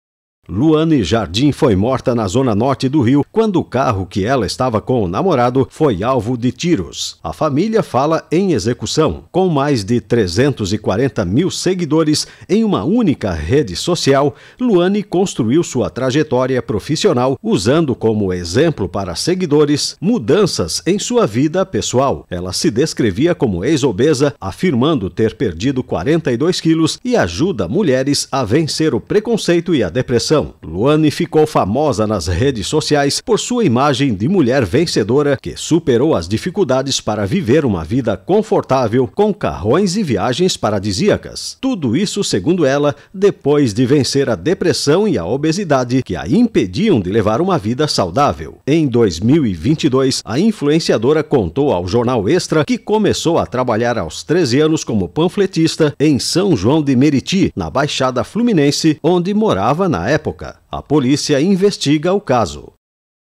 Influenciadora é morta a tiros no Rio de Janeiro
Luanne Jardim foi morta, na Zona Norte do Rio, quando o carro que ela estava com o namorado foi alvo de tiros. A Família fala em execução.